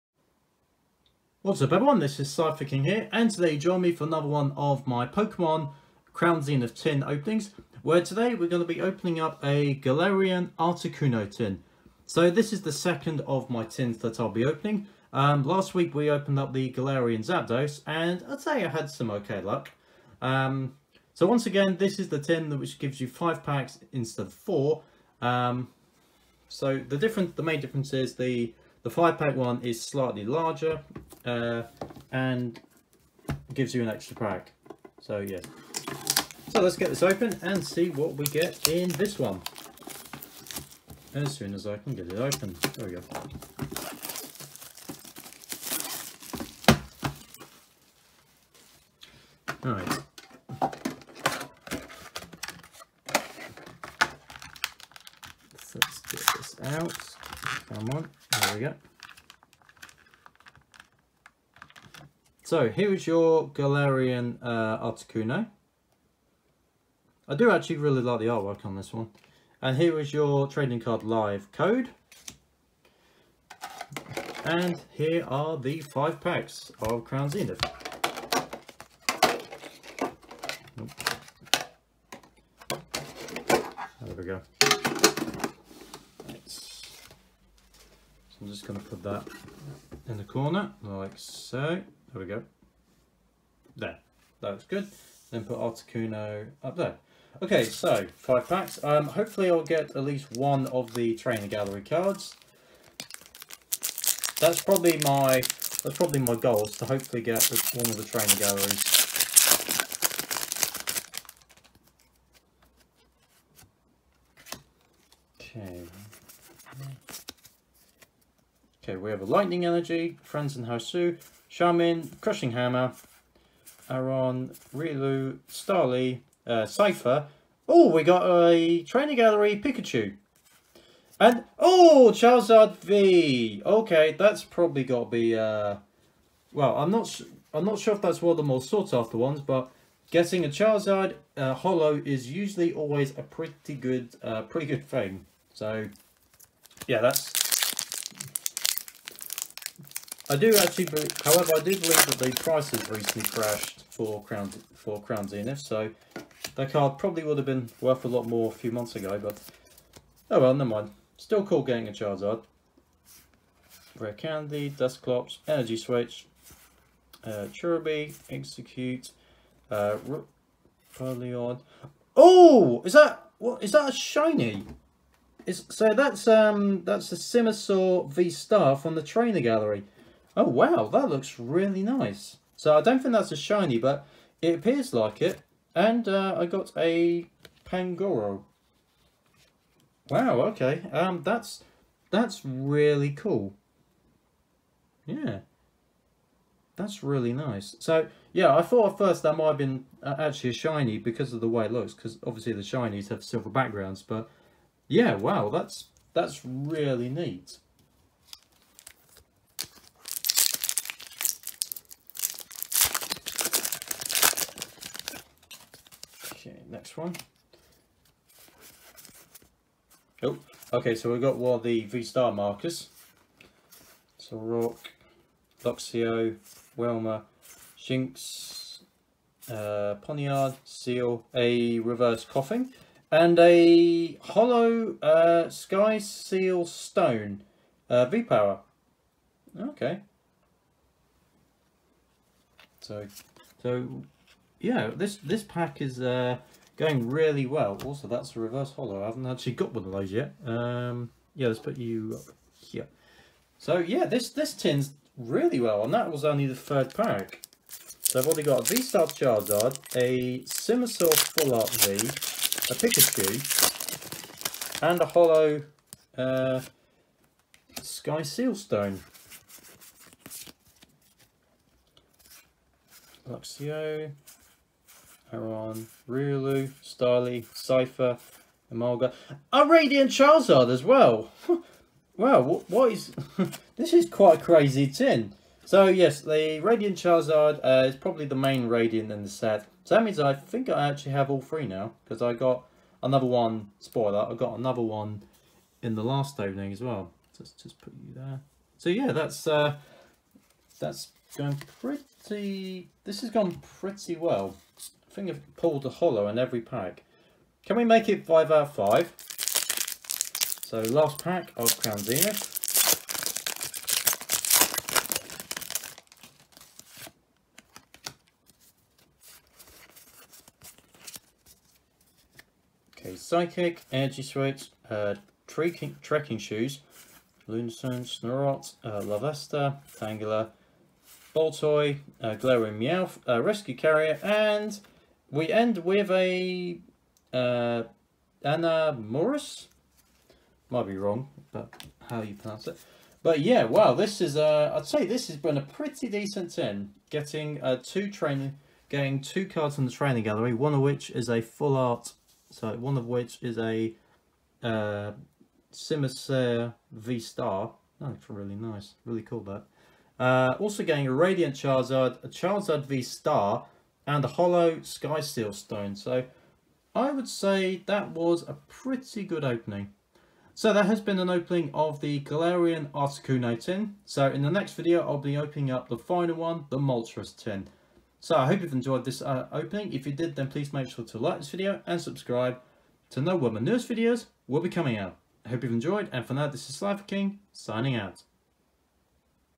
Pokemon Crown Zenith Galarian Articuno Tin Opening
This is Opening Video in which I open a Pokemon Crown Zenith Galarian Articuno Tin.